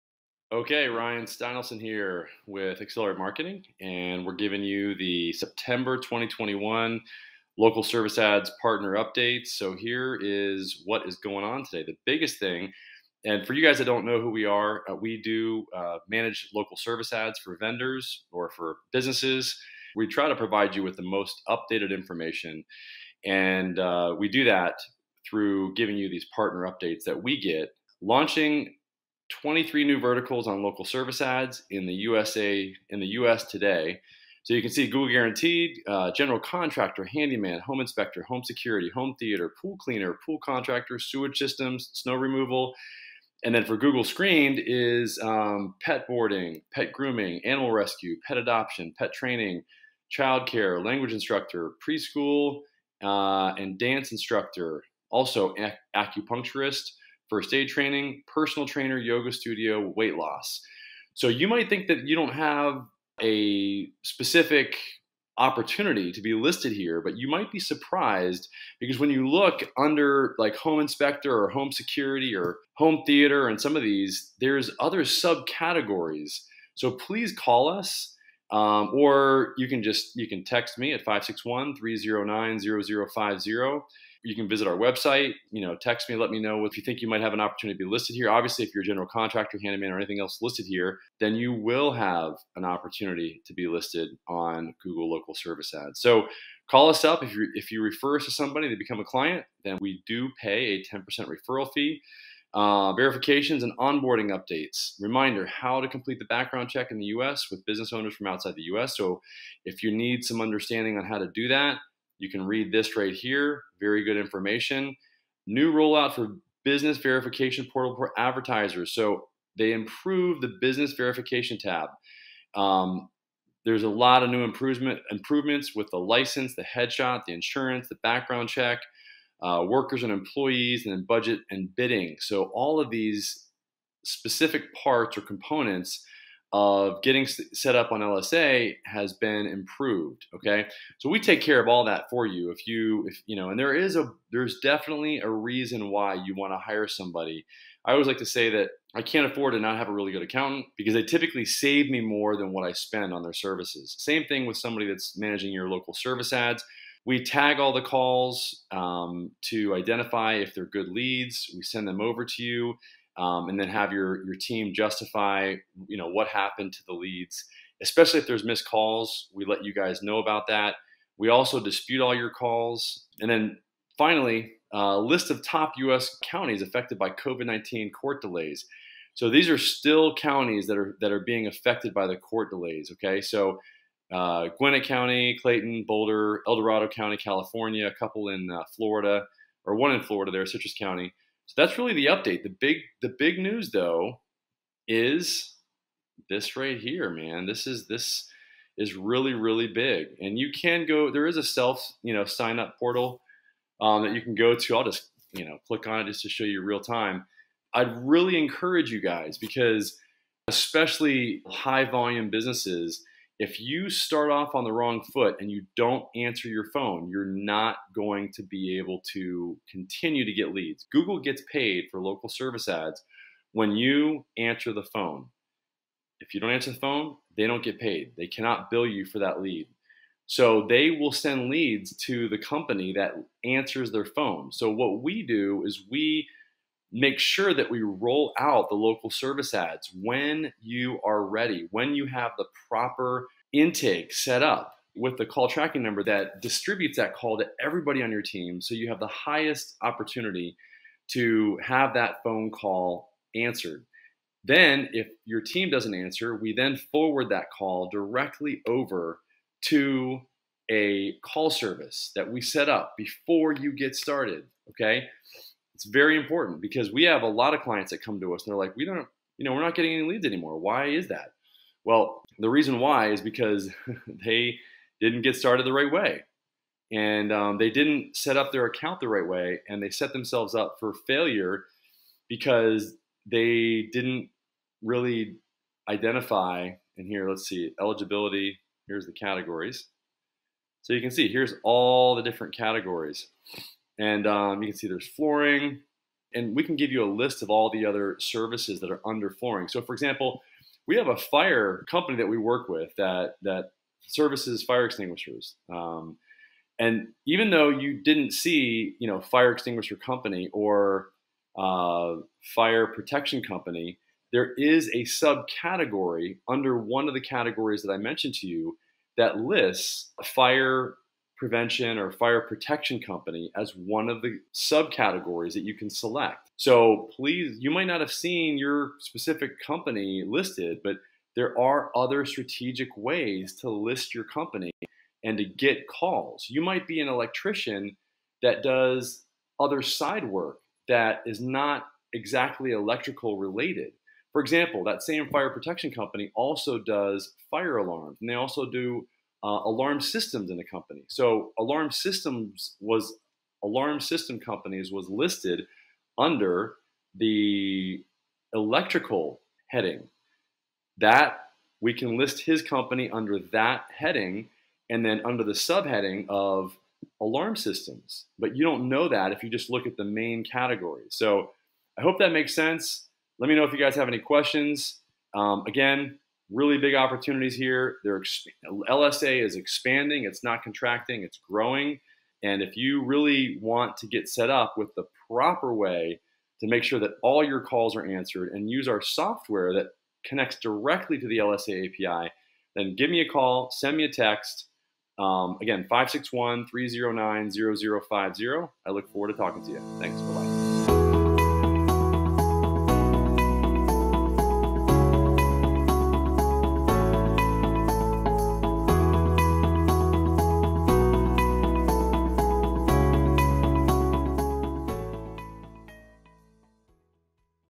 Local Service Ads Partner Update: Level up your Local SEO!
Ryan Steinolfson from Accelerate Marketing briefly goes over the Local Service Ads Partner Update from September 2021.

Chapters:
0:00 Introduction to what's new on Local Services Ads
1:12 Google Screened and Google Guaranteed New Verticals
1:43 Background Checks and Verification Updates
2:54 What counties are affected by court delays due to Covid-19 in the U.S.A?
3:12 The Big News and Recap